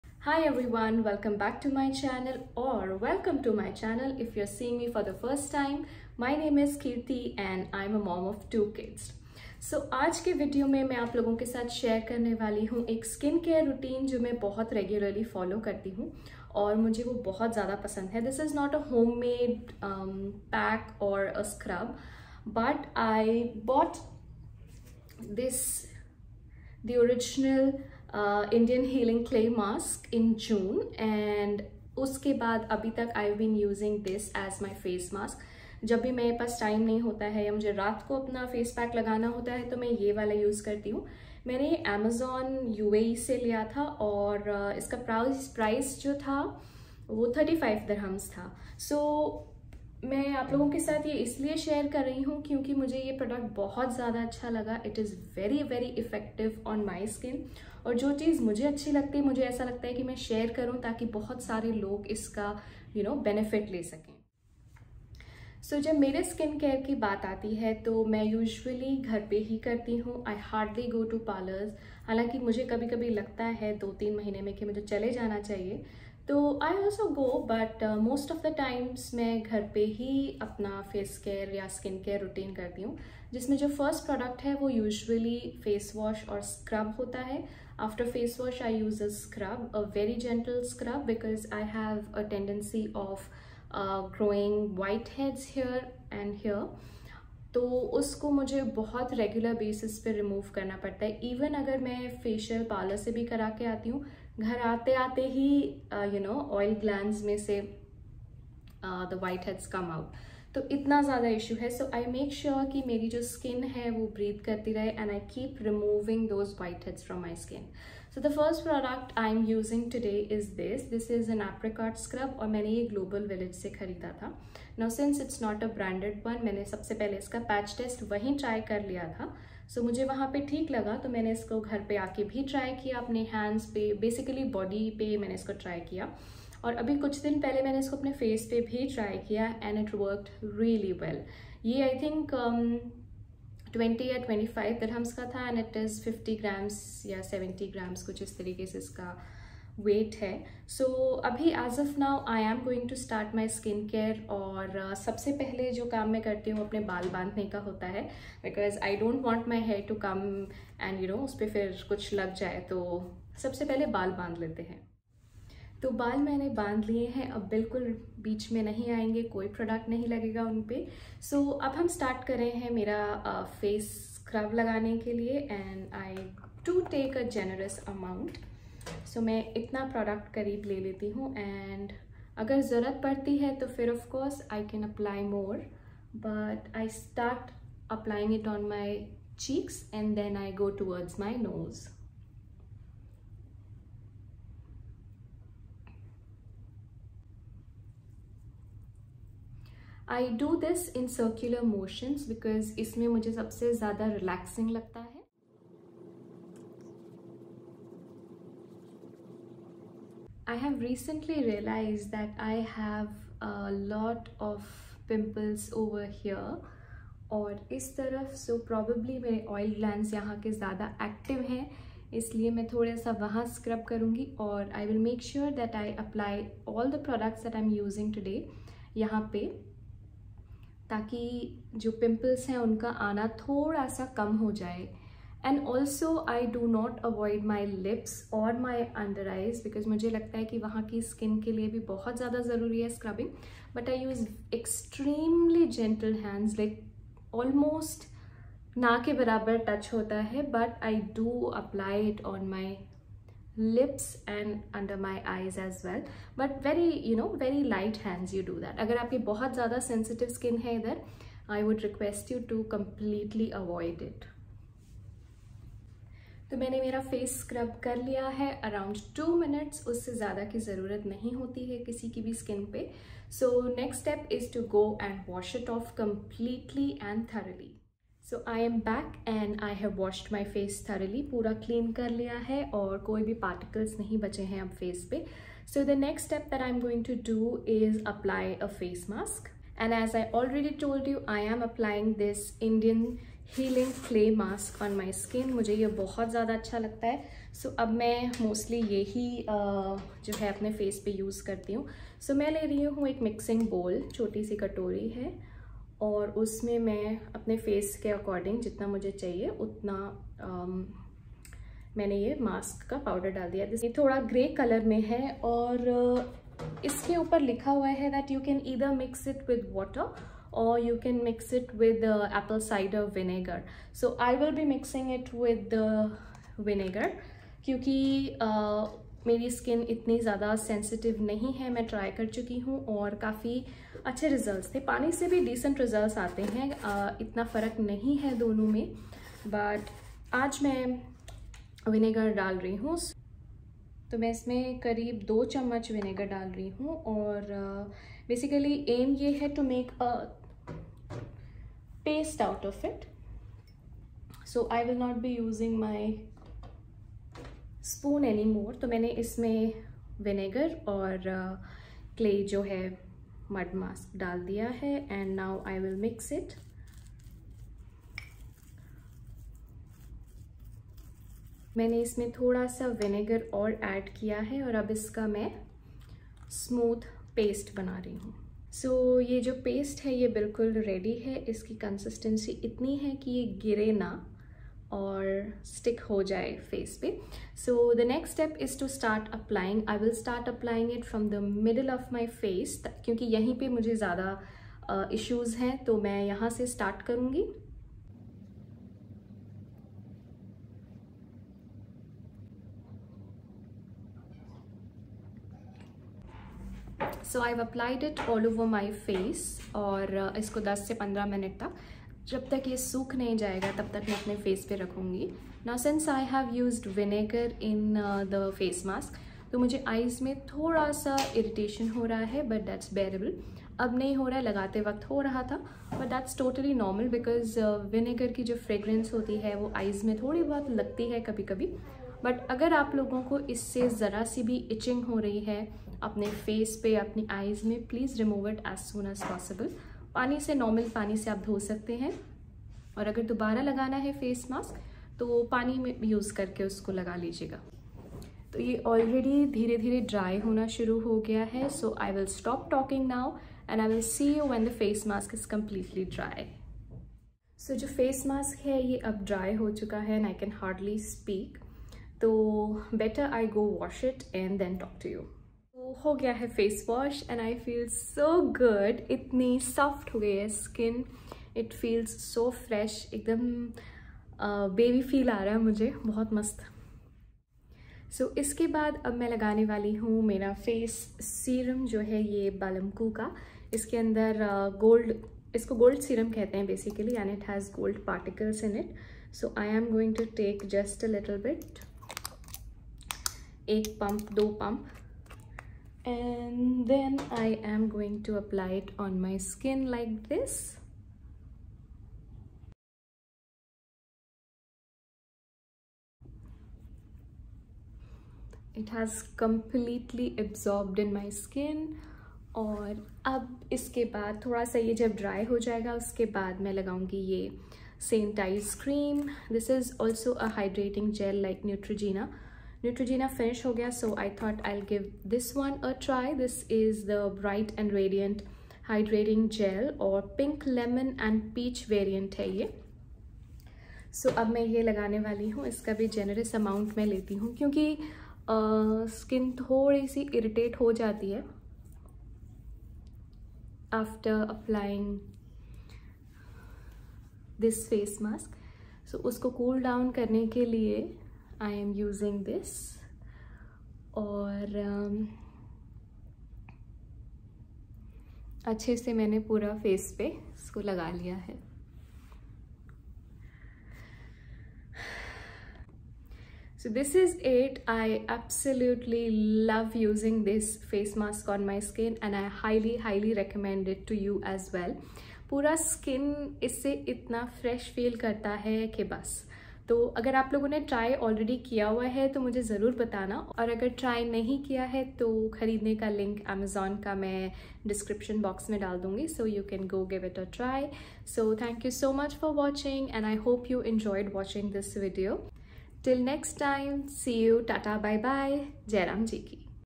Indian Healing Mask Review
Hey all

I’m sharing with you my honest review for the Jadole Naturals The Original Indian Healing Clay.

Here is the Amazon link
Indian Healing Clay The Original Deep Pore Cleansing, 1 Pound by Jadole Naturals